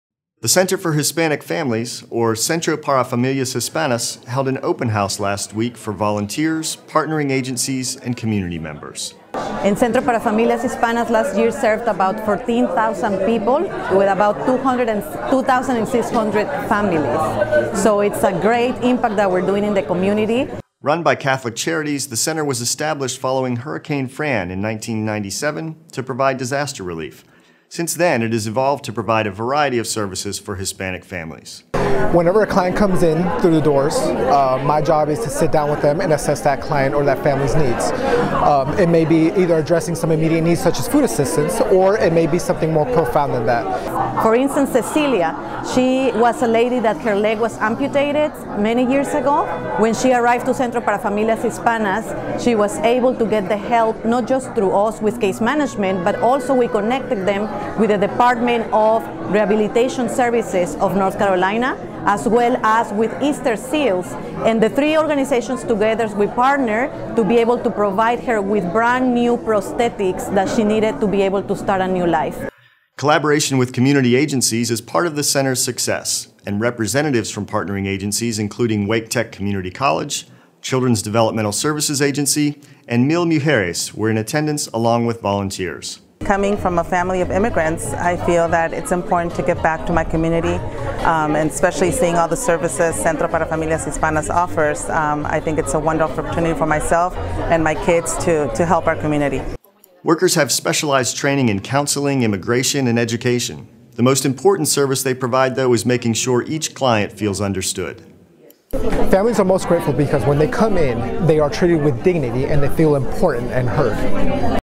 2018 Centro para Familias Hispanas Reunion de Amigos
On Thursday April 20th, Centro para Familias Hispanas held its second annual “Reunion de Amigos” at their facility located on 2013 N. Raleigh Blvd in Raleigh, NC. About 60 people attended the event along with representatives from different partner organizations.